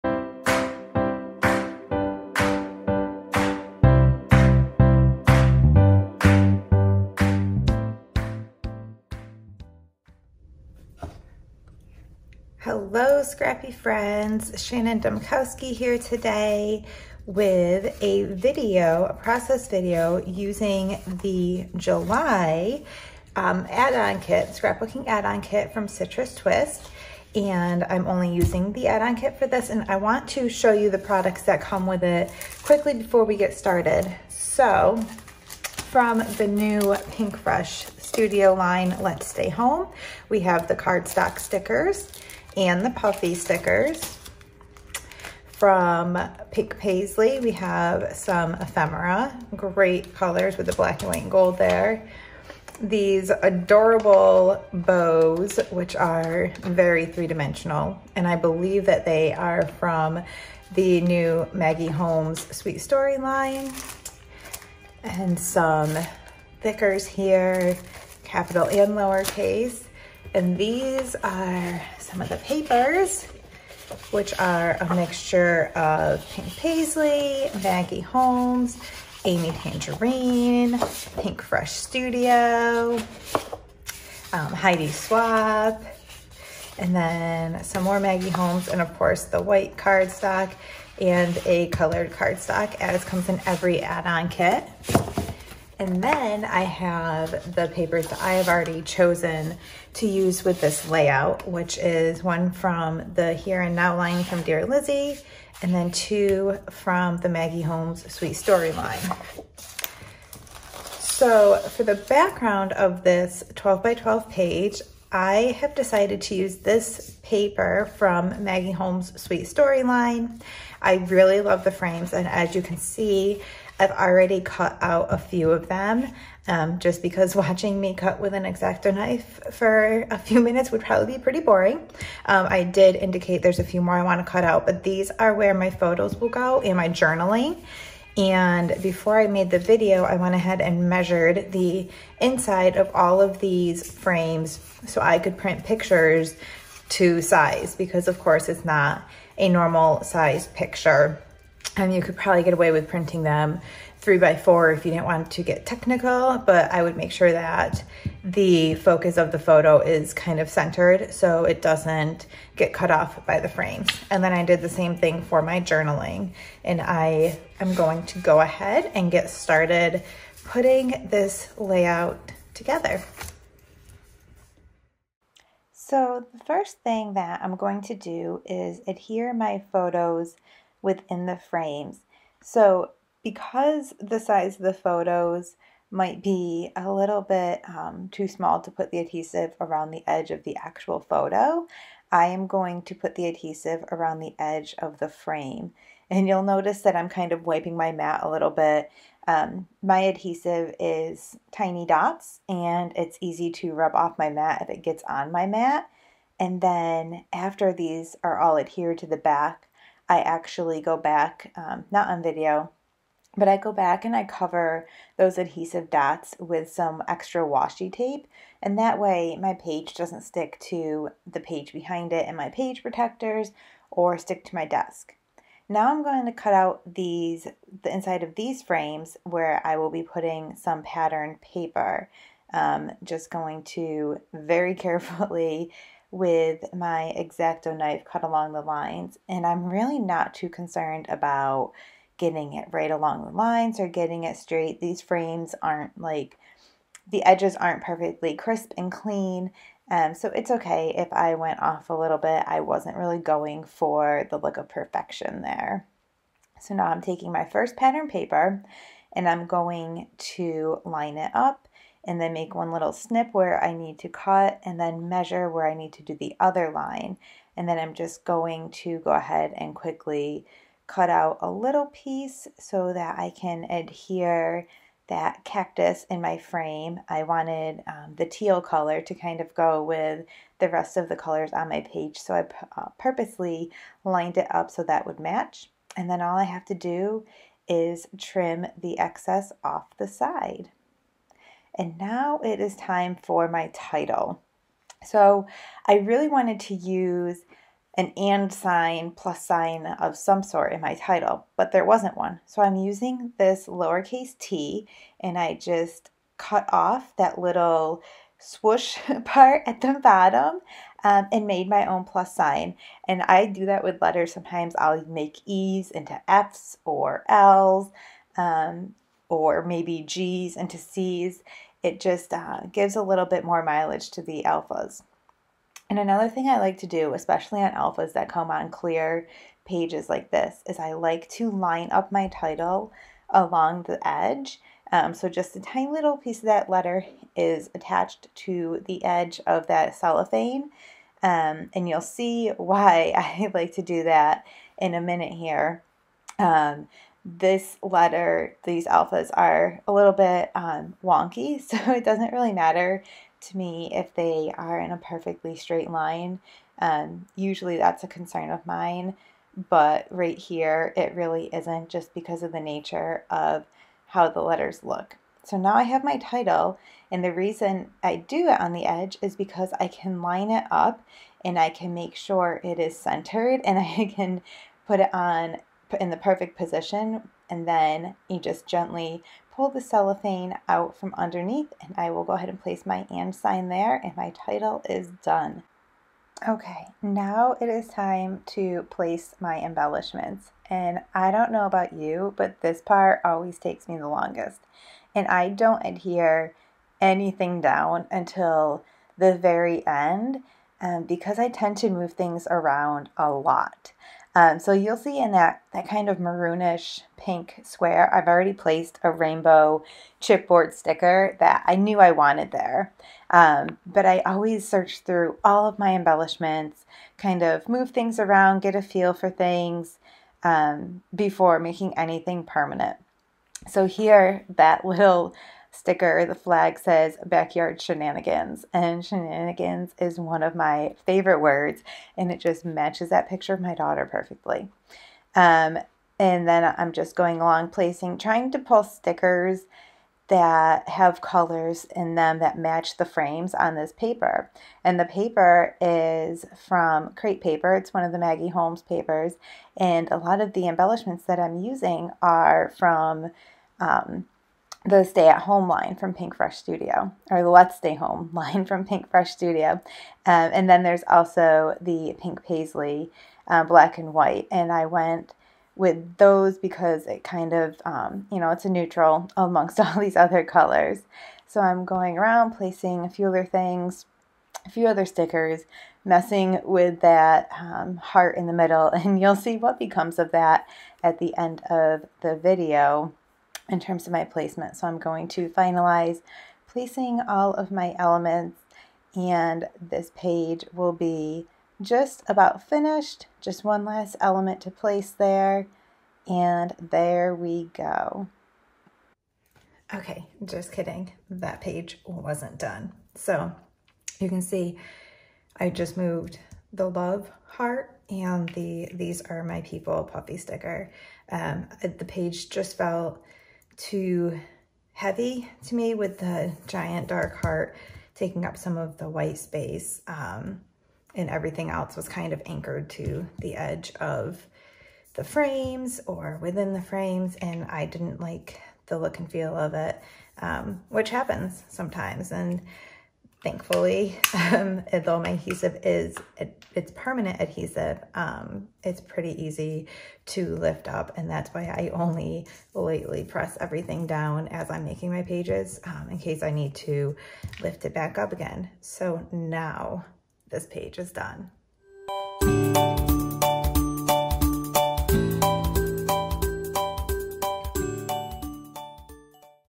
CITRUS TWIST KITS 12x12 Page Featuring the July Add-On with Shannon
Join Shannon as she puts together a 12x12 layout using the Citrus Twist Kits July add-on kit.

Craft a Life you love!